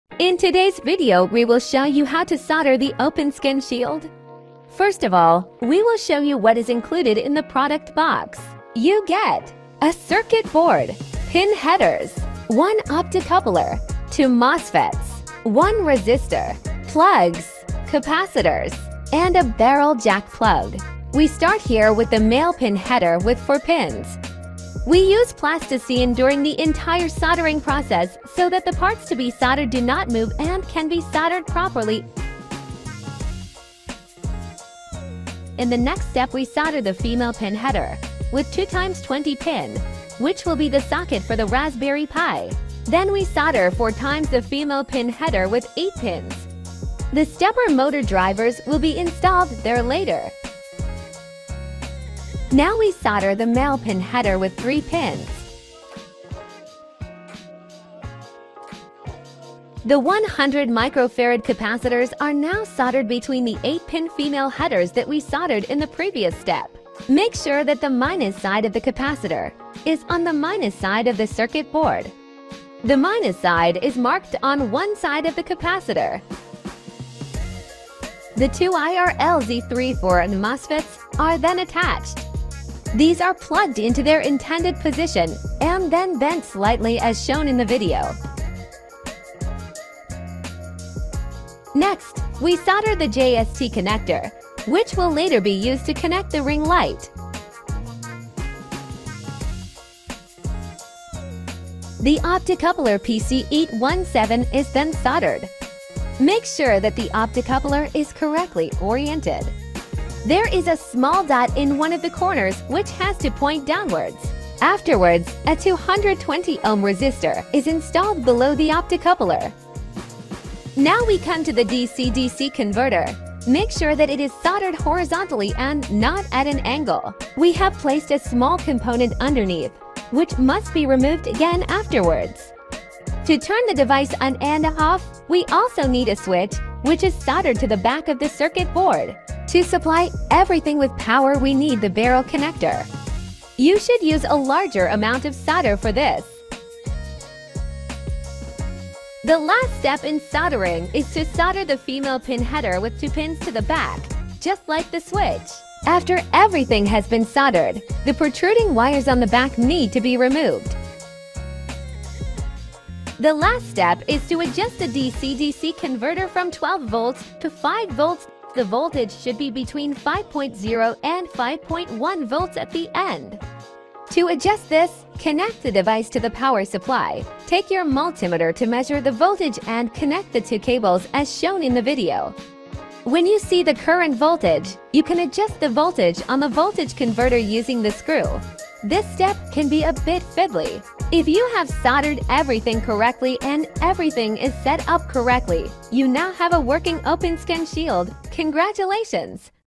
[Tutorial] Soldering of the DIY OpenScan Pi Shield
A step-by-step tutorial on how to solder the Raspberry Pi shield, which is used in our 3D Scanners